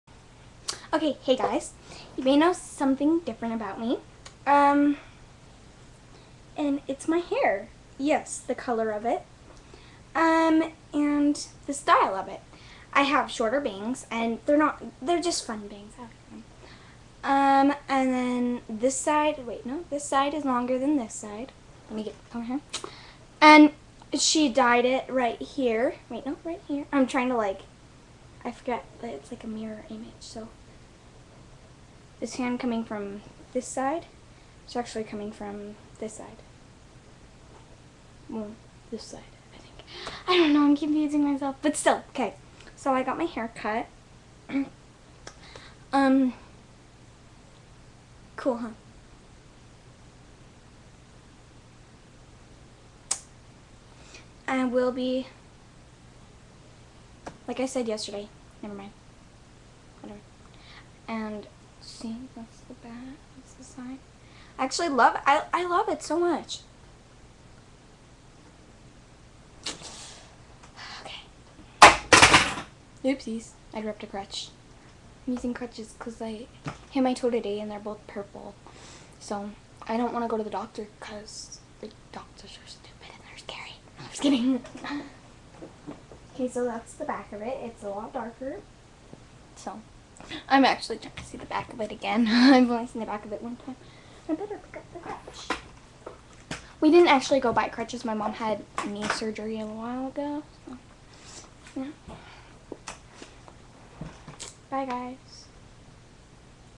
i got my hair colored and cut! its soooo cute :)
yah i love it :)